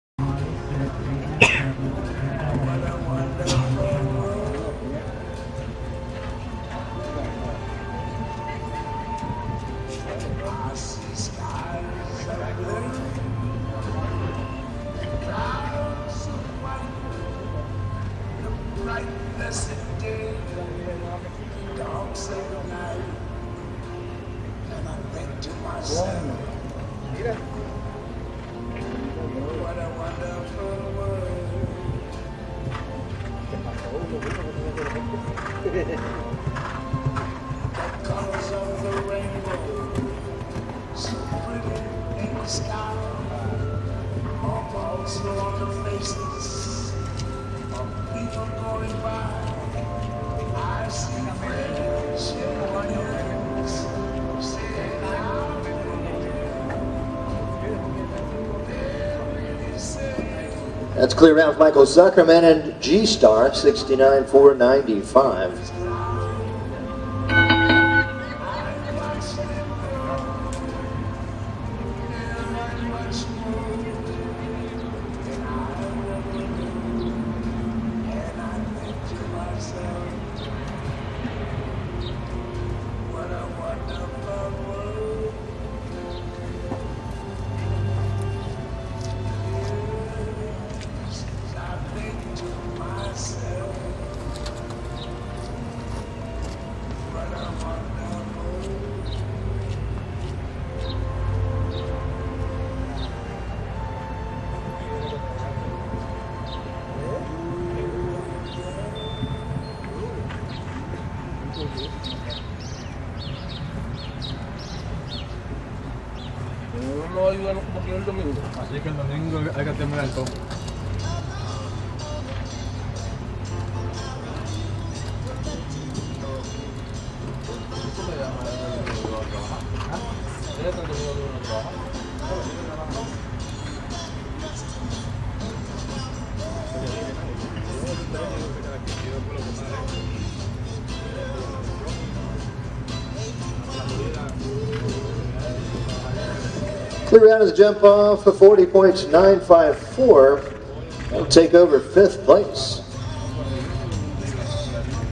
Video of G-STAR ridden by MICHAEL ZUKERMAN from ShowNet!
G-STAR ridden by MICHAEL ZUKERMAN at Old Salem May II.  This round is from class 421, Open Jpr 1.30 m II2b and finished 5 of 56.